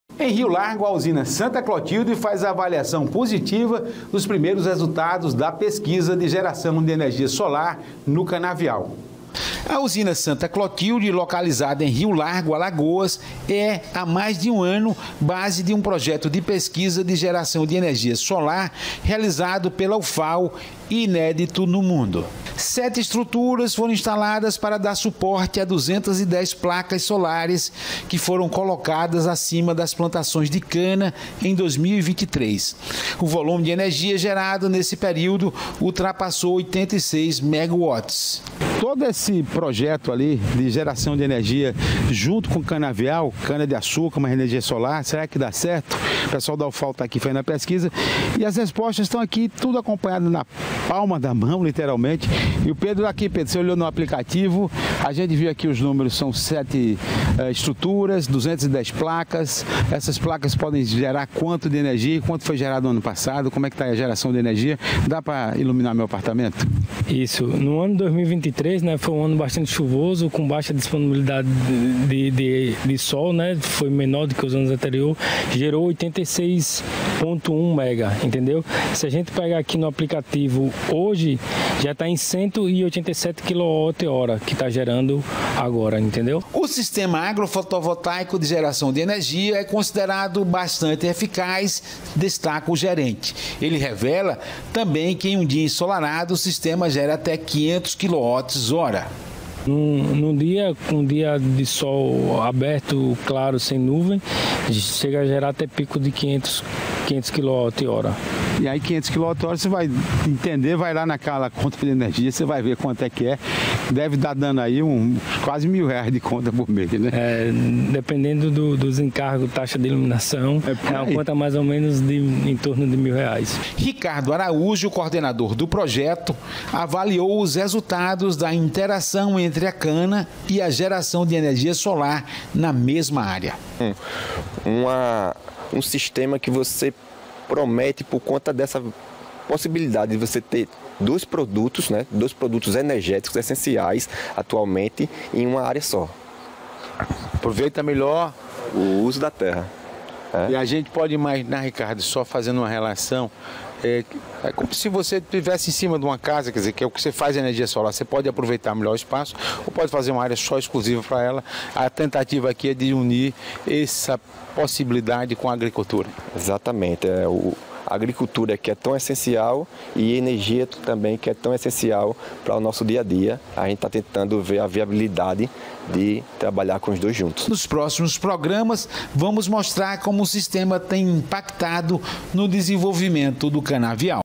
Usina Santa Clotilde faz avaliação positiva nos resultados da pesquisa de geração de energia solar
Todo o projeto de geração de energia é de parceria com a UFAL.